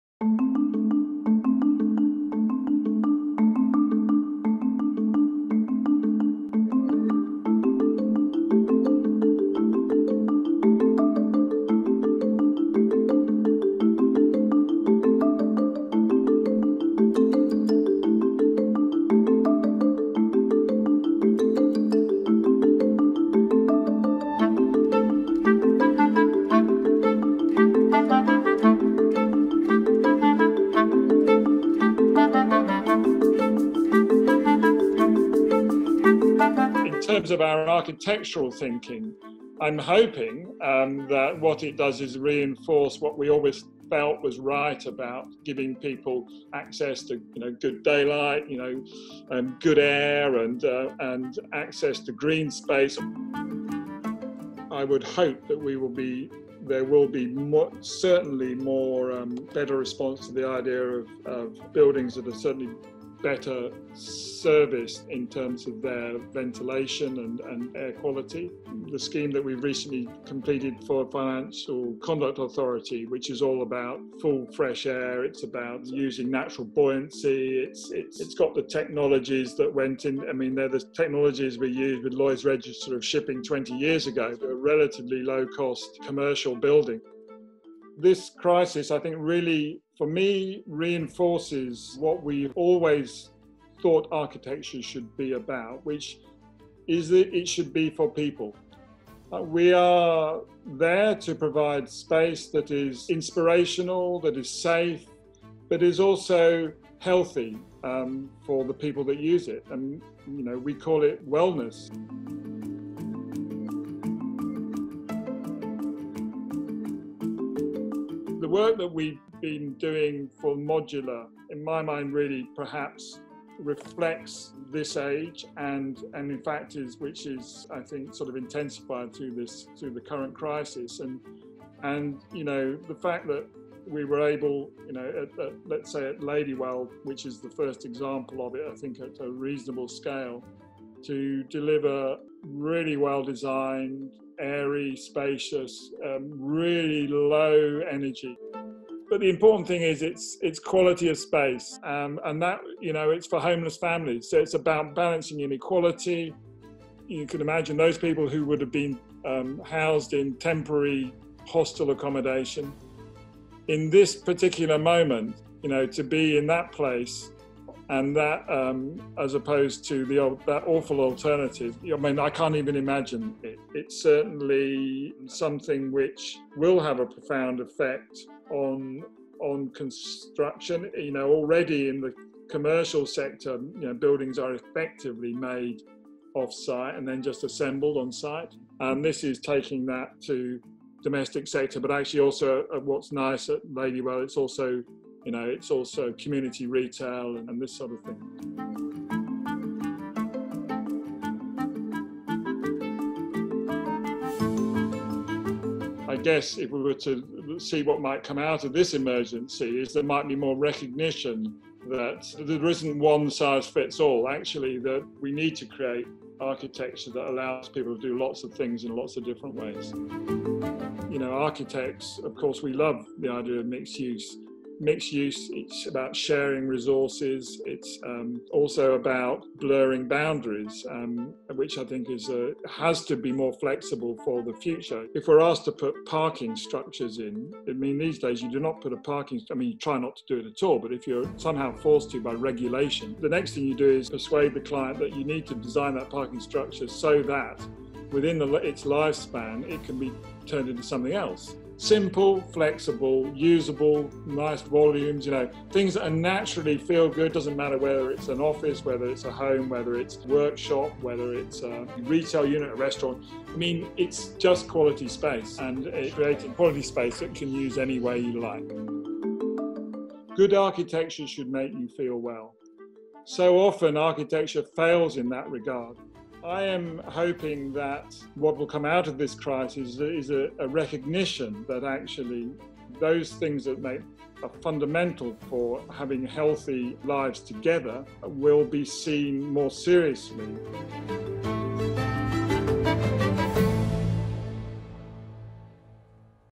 Urban futures: How COVID-19 could reshape architecture
With global health crises becoming an ever-greater threat, how can we pandemic-proof our buildings amid COVID-19? We asked veteran architect and RSHP partner Ivan Harbour about the future of architecture.